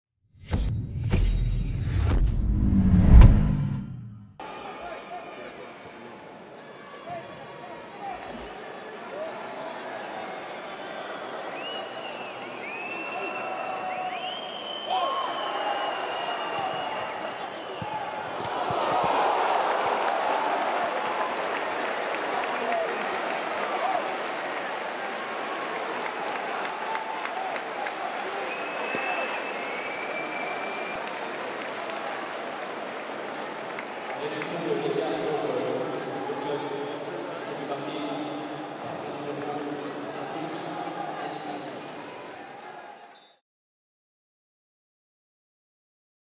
GF38TFC Le ciseau signé Janis Antiste face à Grenoble, plus beau but de la 2ème journée de Ligue 2
Nous aurions préféré un résultat différent dans ce Grenoble/TFC mais malgré tout, félicitations à notre pitchoun Janis Antiste pour ce premier but chez les pros.

🥇 Élu plus beau but de la 2ème journée de Ligue 2 BKT par les twittos.